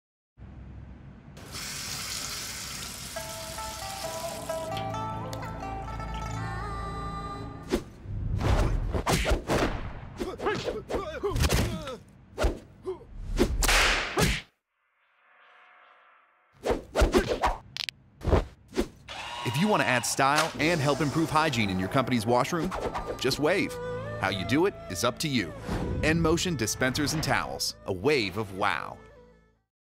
enMotion® Automated Touchless Towel Dispenser The Black Belt
If you want to boost hygiene, just wave. (How you do it is up to you.) Check out The Black Belt in action.